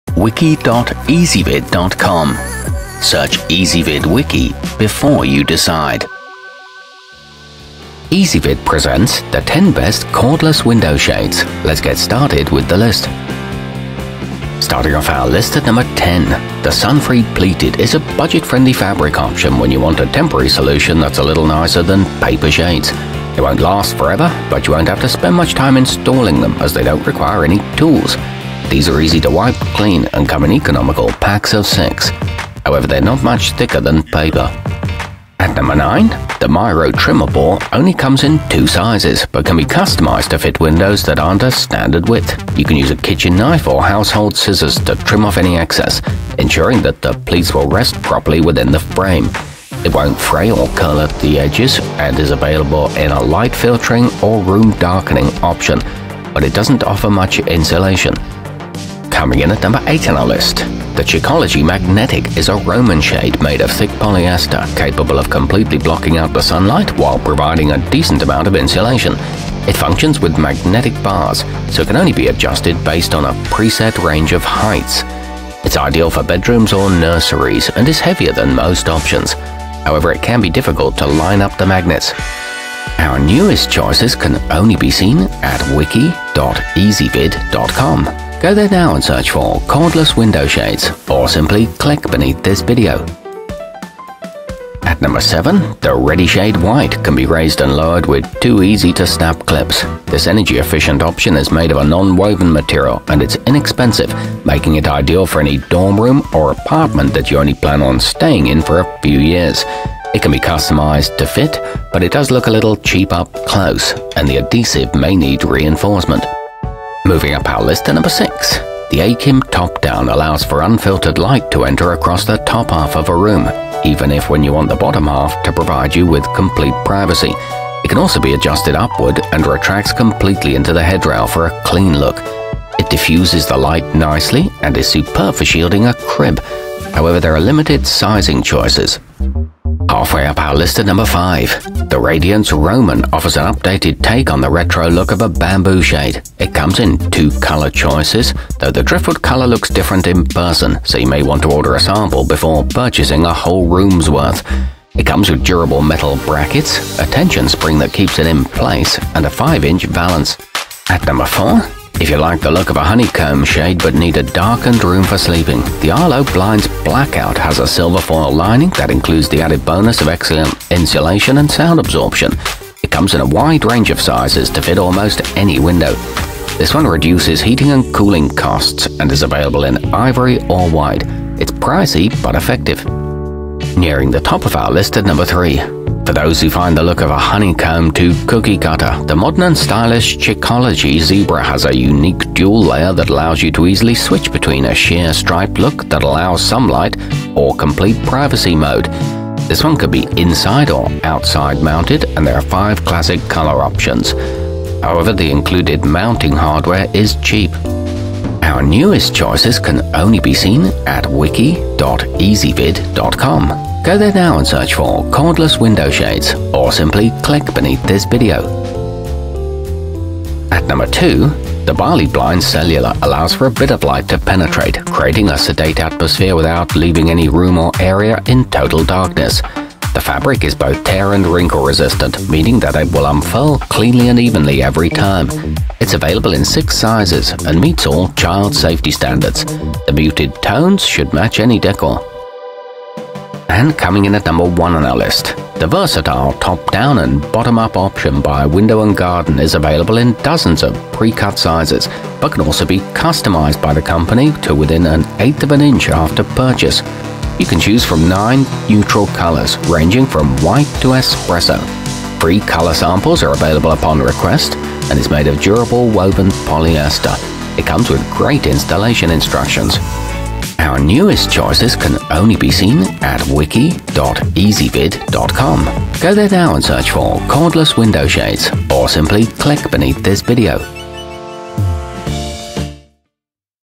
10 Best Cordless Window Shades 2018
Our complete review, including our selection for the year's best cordless window shade, is exclusively available on Ezvid Wiki.

Cordless window shades included in this wiki include the redi shade white, radiance roman, window and garden, chicology zebra, bali blinds cellular, arlo blinds blackout, sunfree pleated, achim top down, miro trimmable, and chicology magnetic.